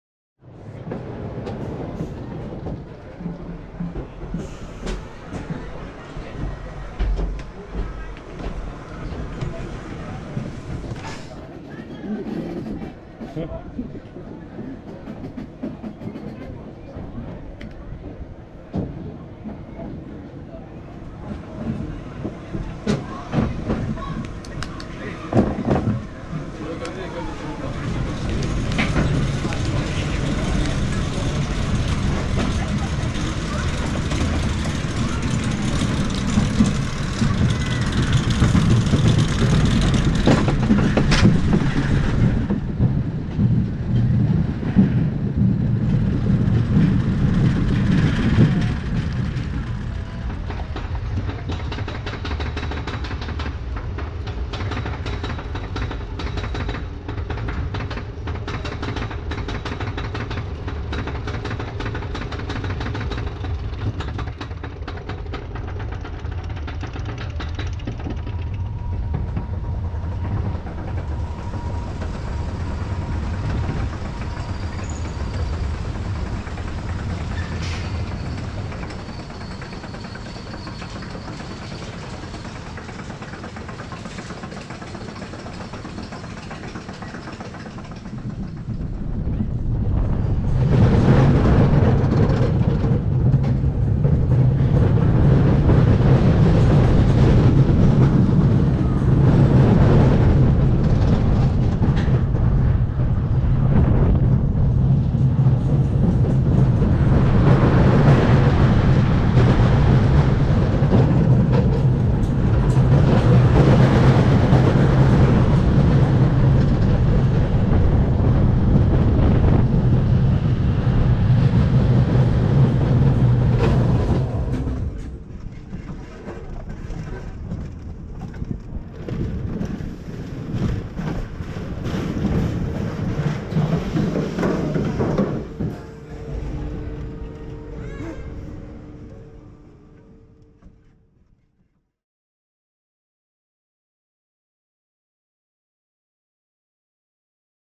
Luna Park Sunny Beach Roller Coaster
POV Video from the Roller Coaster at Luna Park in Sunny Beach Nessebar Bulgaria.